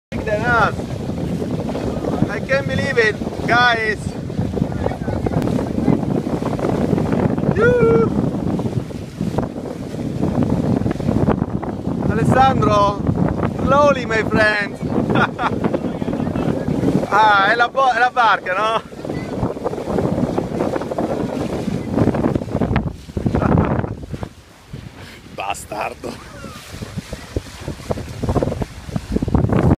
What to do in Catania in one day. Book a sailing tour without any doubt
Sailing with love. Sailing with Sicilians! This is an amazing way to enjoy Catania by the sea. If you are in Catania just for one day. Gaetano will let you enjoy a full day on his boat or just the sunset. The tour is the best ay to discover the beaches of Catania, Acitrezza Bay and Medieval Castle of Acicastello, by a private spot .